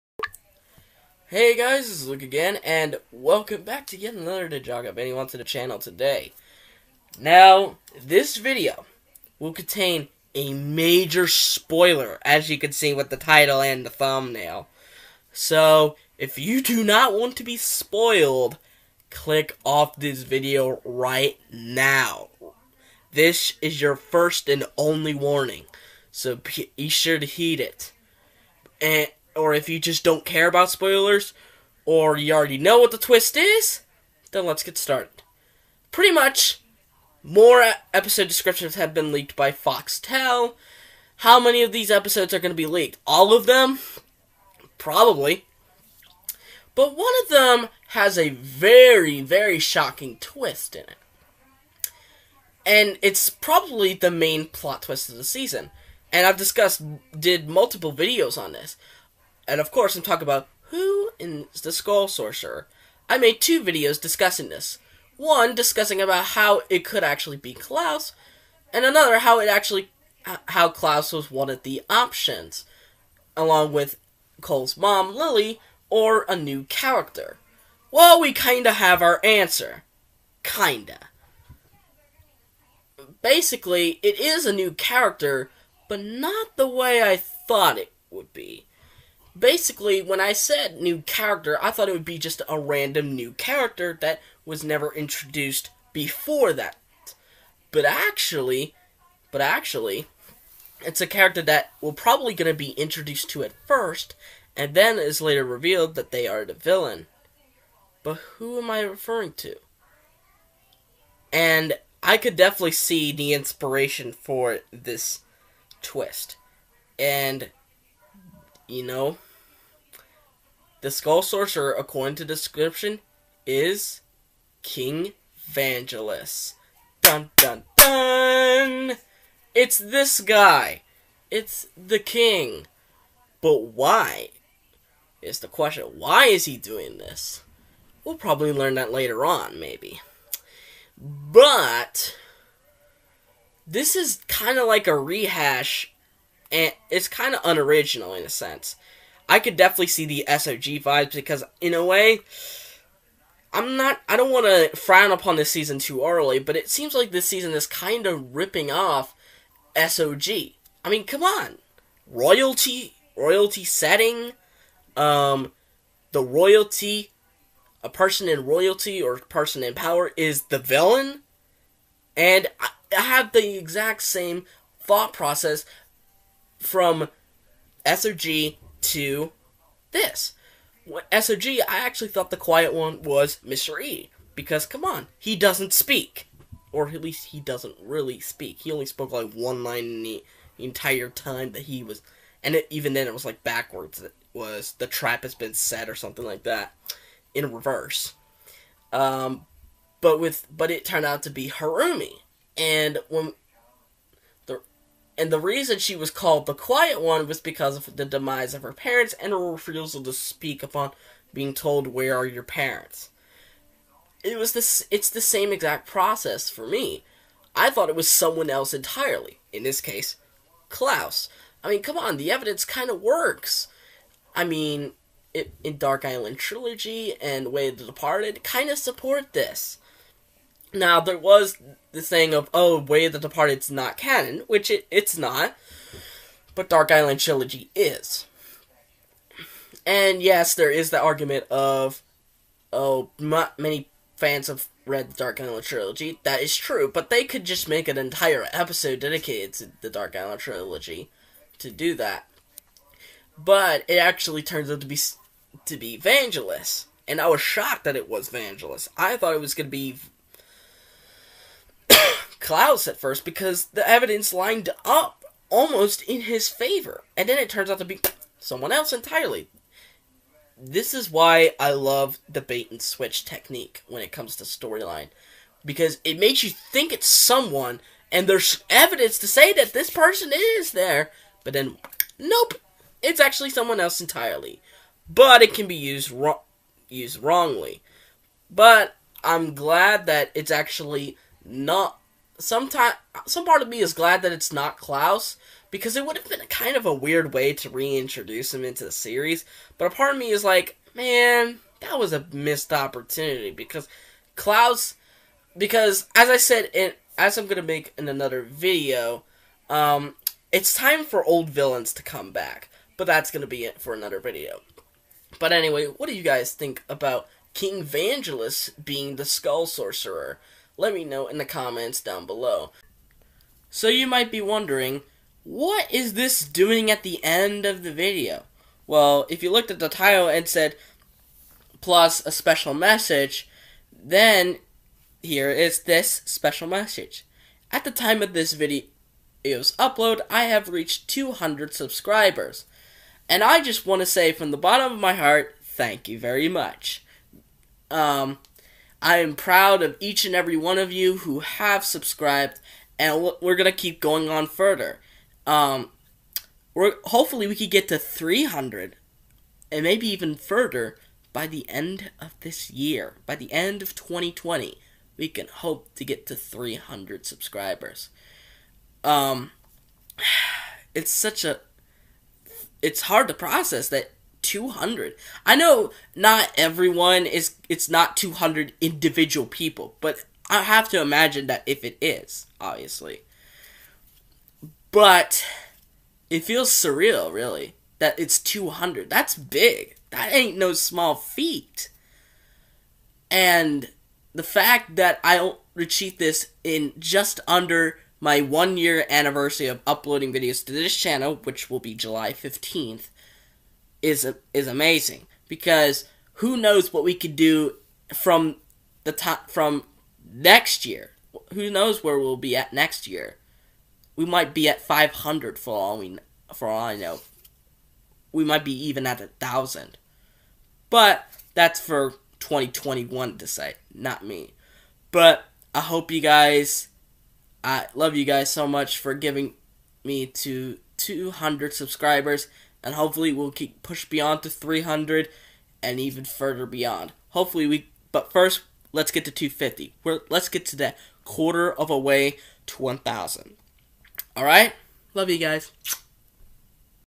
(MAJOR SPOILER!)The Skull Sorcerer's Identity has been revealed! + A Special Message From Me To You!
This one has a MAJOR SPOILER ALERT so if you do not want to know who the Skull Sorcerer is, please click off of this video RIGHT NOW!

Episodes of S13 have been getting their plots leaked left and right and this one included the true identity of the Skull Sorcerer and it's not who you think it is! Who is he?

I also reached 200 subscribers today so after the identity reveal ends, I will give a special thank you message from me to you all who have been with me throughout this YouTube journey!